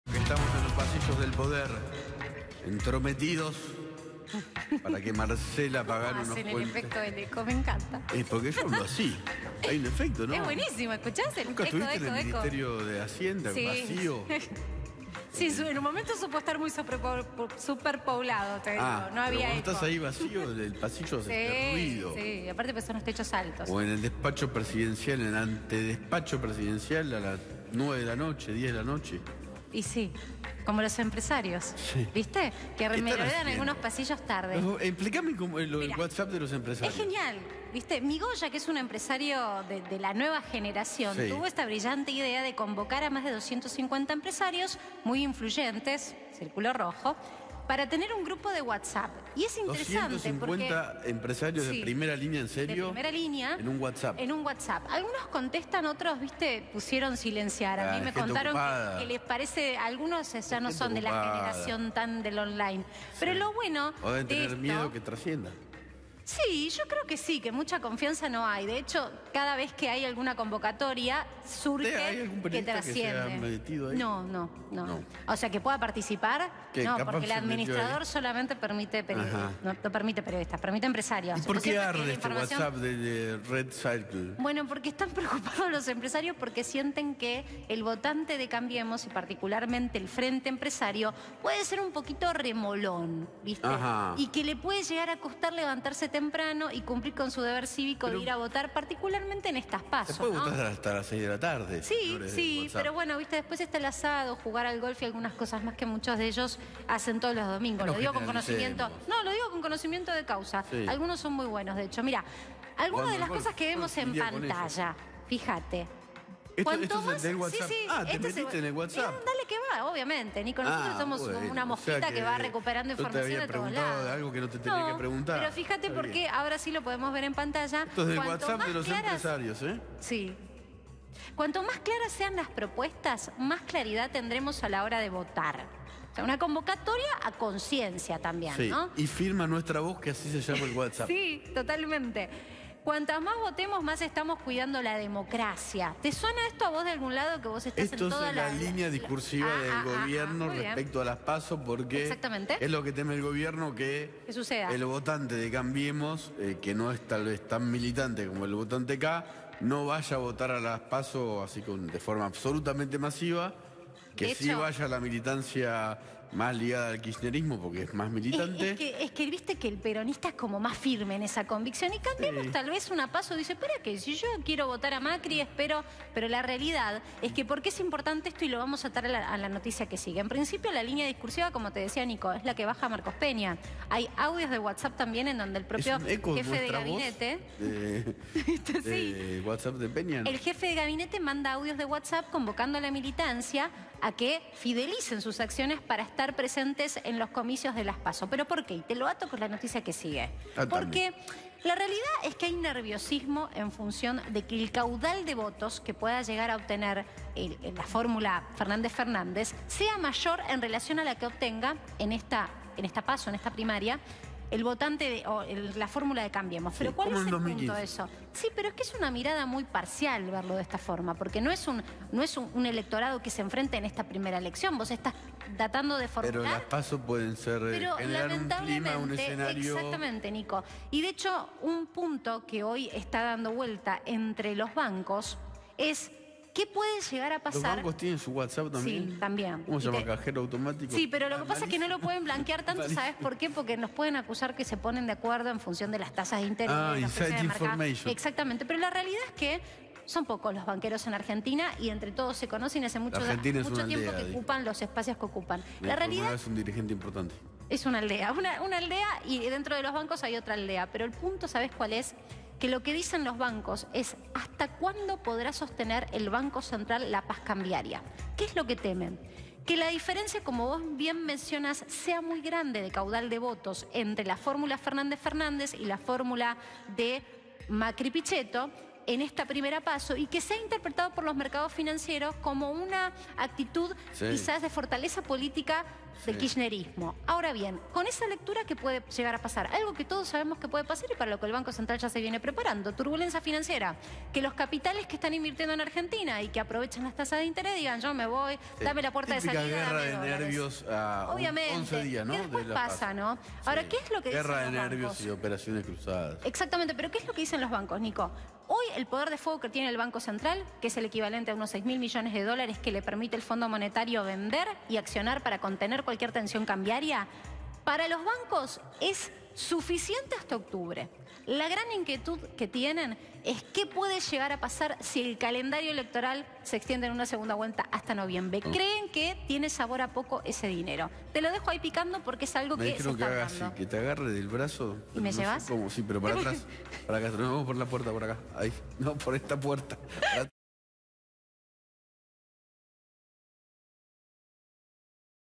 Secretos mal guardados: los pasillos del poder al desnudo
Nicolás Wiñazki y Marcela Pagano revelan los secretos de los pasillos del poder.
Arde "Nuestra voz", el WhatsApp que crearon los empresarios: qué se dice en el epicentro del círculo rojo.
Los audios de Marcos Peña a la militancia de "Juntos por el Cambio".
Dólar quieto hasta octubre... ¿y después? Bancos el alerta